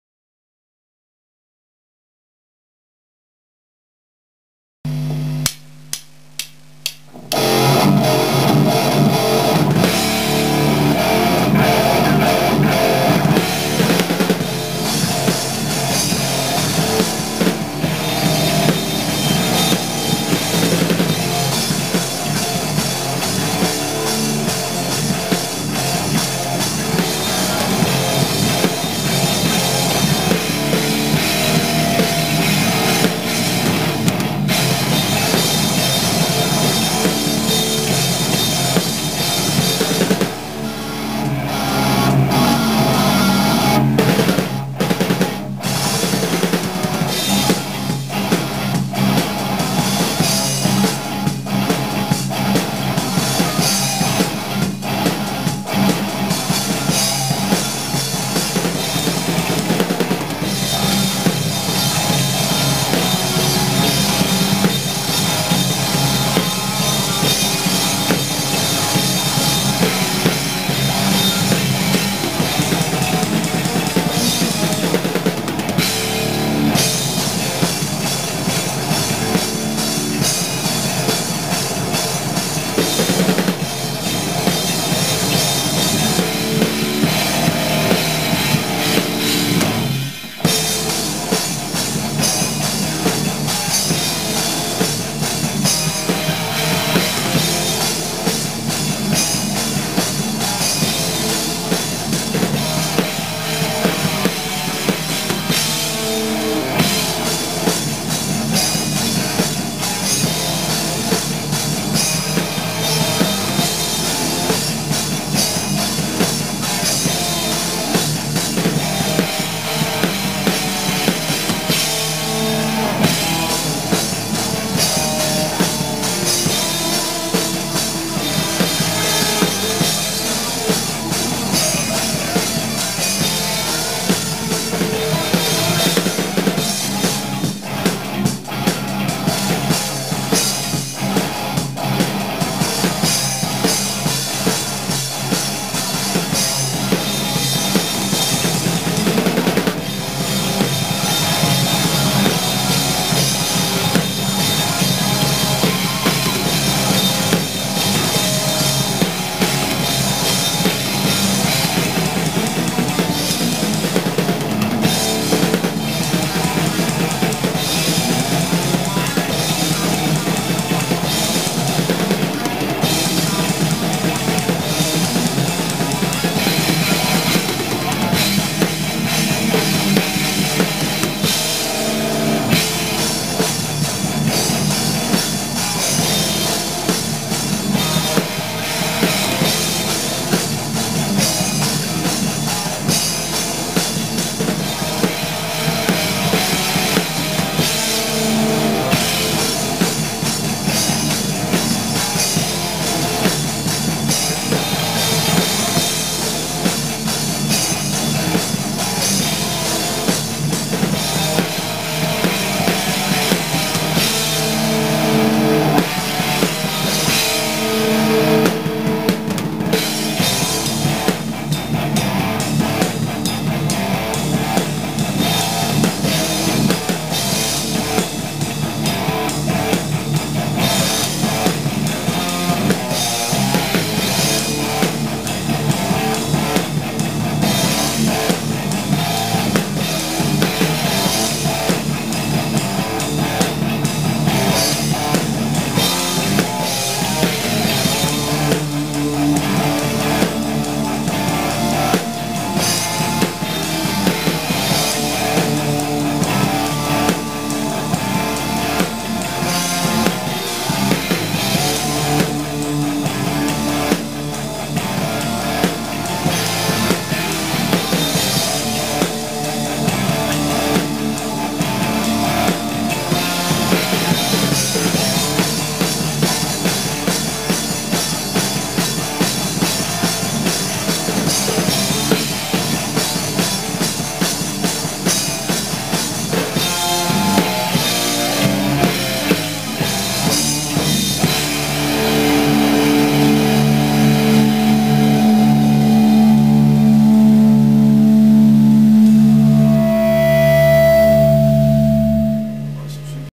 Dead Man Walking - drums by Kris
Performance by Dead Man Walking drummer Krzysztof Pilch 'Kris'. Kris bands are: EndGame, Dead Man Walking, Electric Hate Machine. I use ddrum dominion drums,Sabian B8 cymbals and Sonor twin pedal.